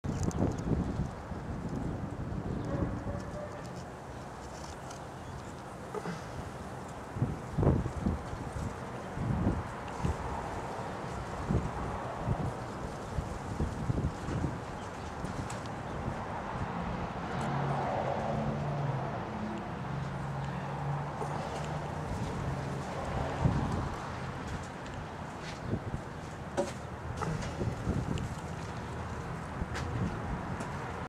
Newborn Black Widows making Web Parachutes P3
Micro-zoom HD video of little Black Widow babies creating little silk parachutes to fly away with.